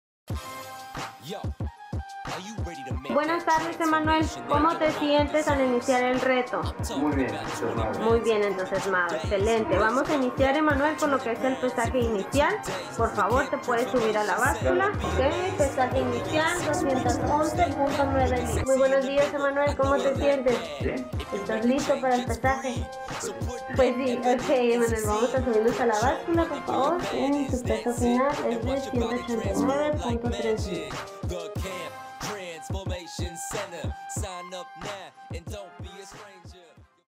Zona Dorada Fitness 6 Week Challenge Results - Emmanuel Durazo
We will now check the result of Emmanuel's 6 week weight loss challenge!